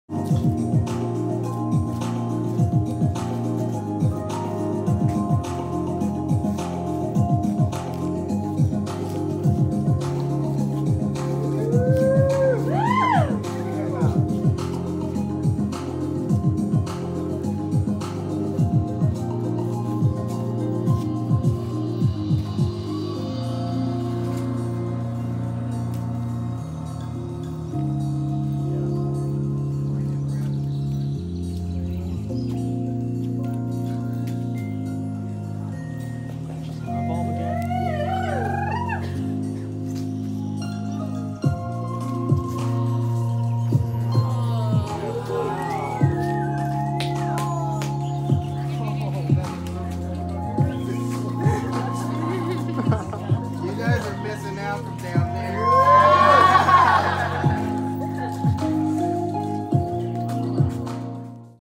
Flight of the Monarch (Aerial View)
Silk will always be my favorite prop to dance with; the way it organically moves in the wind and perpetually blossoms like a vivid flower.